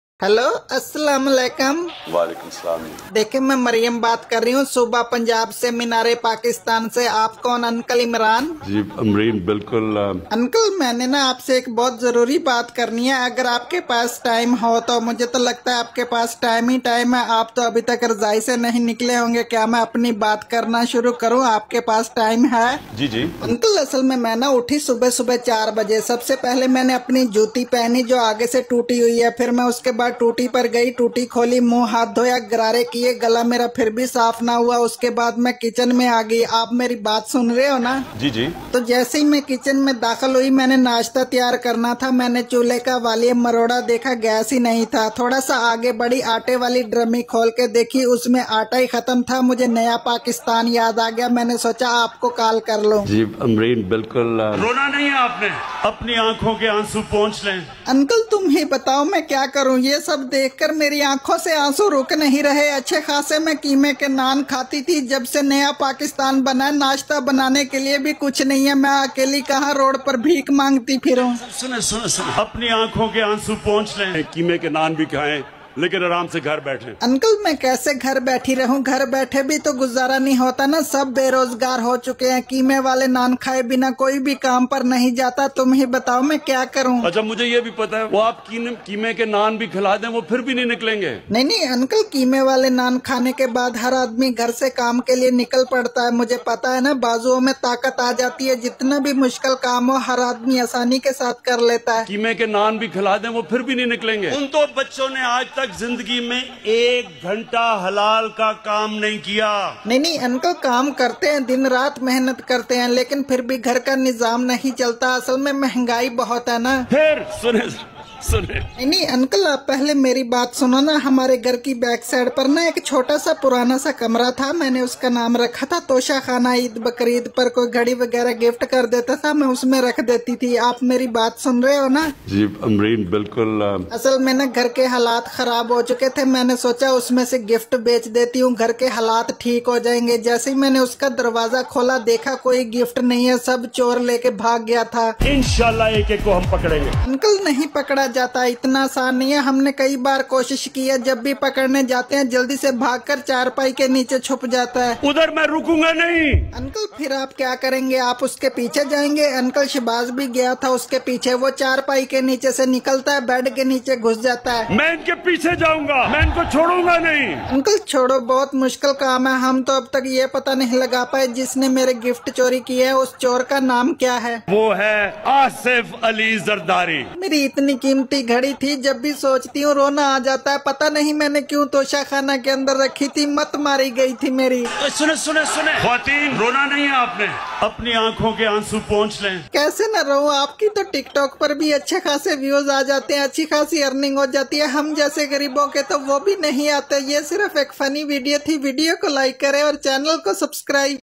Imran Khan and Maryam Nawaz's prank night 😱😱😱
Assalam alekum my YouTube family, how are you? This video is a funny video of Imran Khan and Maryam Nawaz, so you have to watch it completely. rimshavlogs funny $